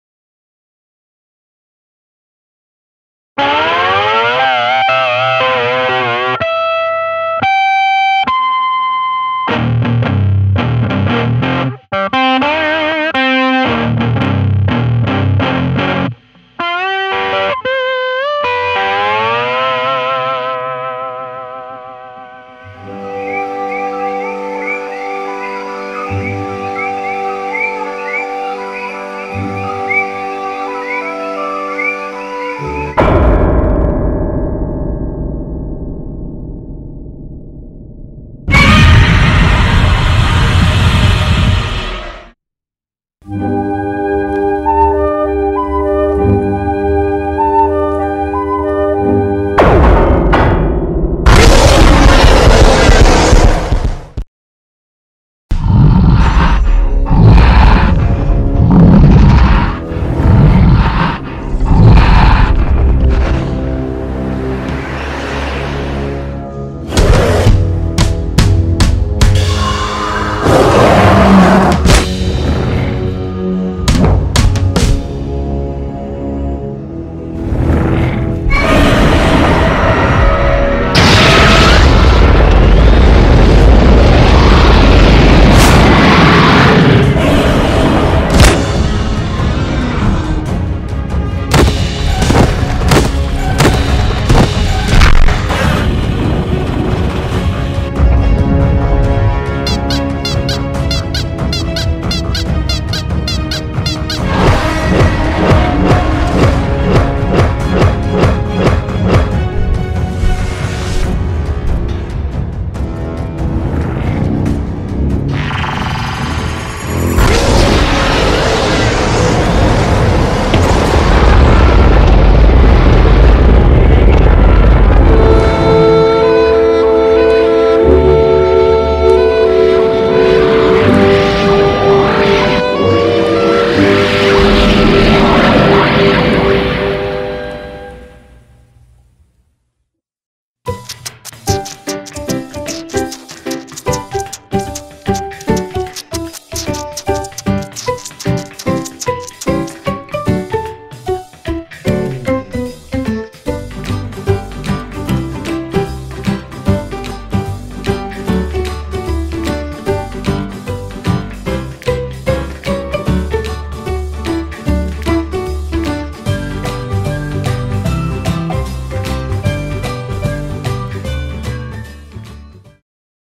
Bambi meets Godzilla meeting King Kong 🔞 [Bermuda triangle Island: Episode 01]
"Bermuda triangle Island" Episode 01:  Bambi meets Godzilla meeting King Kong

On a Island lost in the Bermuda Triangle called "Bermuda triangle Island", two legendary titans are fighting for supremacy. King Kong (2K) VS Godzilla (God-Z). Bambi will be the witness of this epic meeting...

Directly inspired by Marv Newland's "Bambi meets Godzilla" 1969

0:00 Parental advisory
0:04 Opening on slide guitar by Benjamin Hadjeb
0:22 Bambi meets Godzilla
0:42 Bambi meets Godzilla Part 2 (KingKong)
0:54 KingKong VS Godzilla
2:04 Bambi meets Godzilla part 3 (KingKong VS Godzilla)
2:20 Happy end
2:30 Credit, LIKE and SUBSCRIBE to my channel